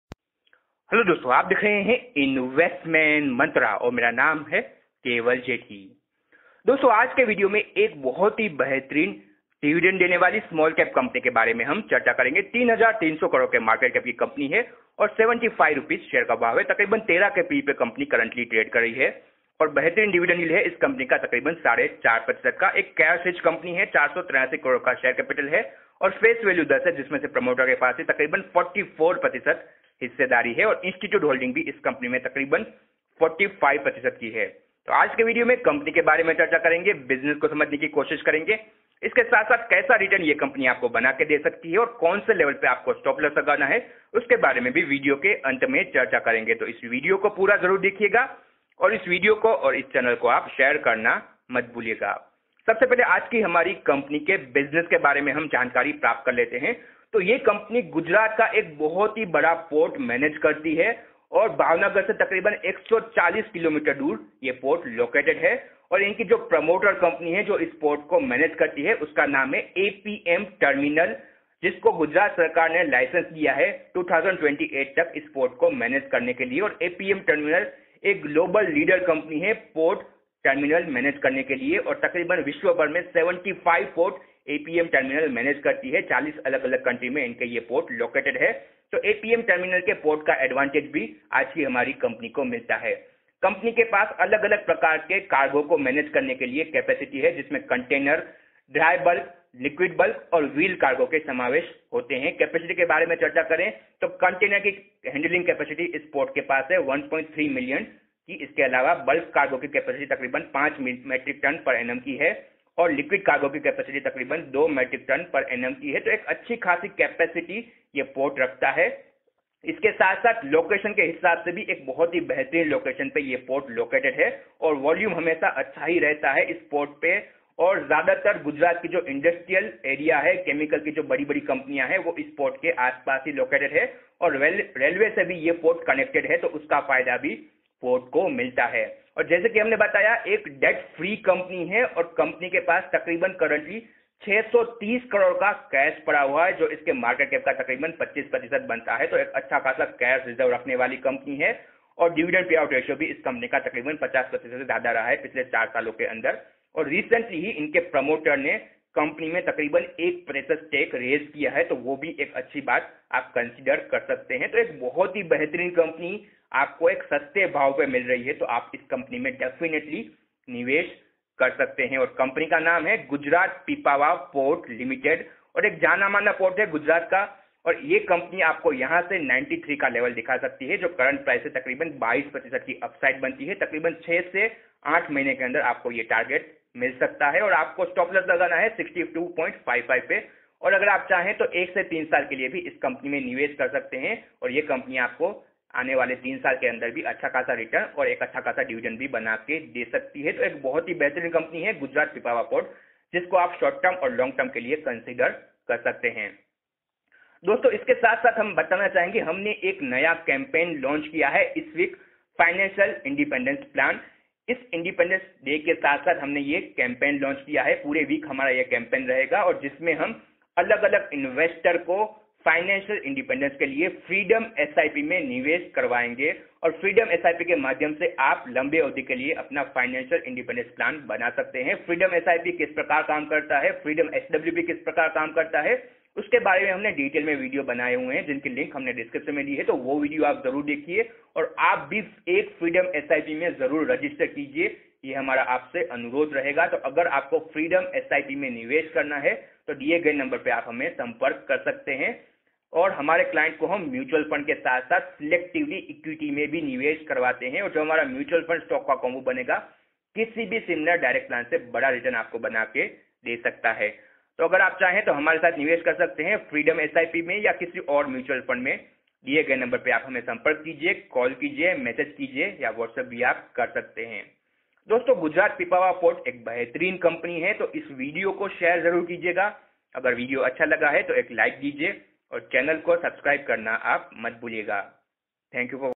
HIGH DIVIDEND PAYING SMALL CAP || PART OF APM TERMINALS || GUJARAT PIPAVAV PORTS
High Dividend paying low Price Undervalued Small Cap Company Gujarat Pipavav Port.
Debt Free company with 630 CR cash on book and high dividend payout.
Own by global port leader company APM Terminal.s